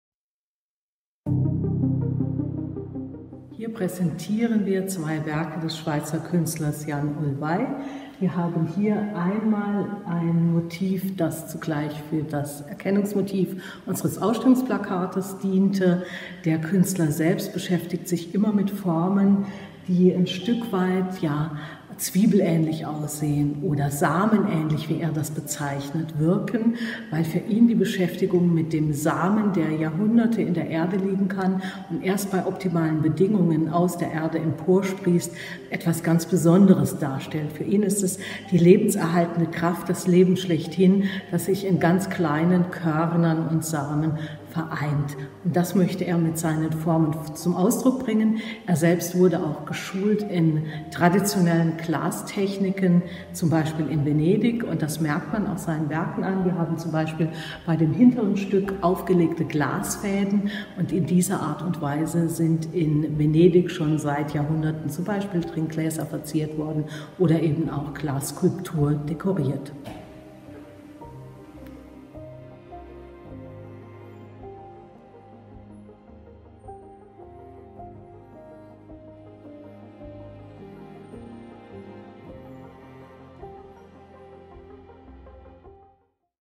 (3/4) CHROMATIK. Klang und Farbe in der modernen Glaskunst / Yann Oulevay, 2012 & 2013
CHROMATIK. Klang und Farbe in der modernen Glaskunst. Glanzlichter aus der Sammlung Peter und Traudl Engelhorn des mudac
Reiss-Engelhorn-Museen Mannheim | verlängert bis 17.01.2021

Kuratorin Eva-Maria Günther entführt in die faszinierende Welt moderner Glaskunst.

Film 3/4:
Yann Oulevay: Graine Topaze (2013) und Graine miroitée (2012)
Die beiden Arbeiten sind typisch für das Werk von Yann Oulevay. Inspiriert von der Natur, fertigt er samenartige Gebilde aus farbigem Glas in unterschiedlichen Größen.

Verbindet man mit dem Material Glas in der Regel Transparenz und Farblosigkeit, so entlarvt die Schau "Chromatik" dieses Klischee und präsentiert 36 Exponate in schillernden Farbtönen. Dabei handelt es sich um Glanzlichter aus der Sammlung Peter und Traudl Engelhorn des mudac/Lausanne. Die Ausstellung vereint Werke von den späten 1960er Jahren bis heute. Es sind Arbeiten namhafter Künstlerinnen und Künstler aus Europa, Japan sowie den USA – von Frantisek Vizner bis Salvador Dalí.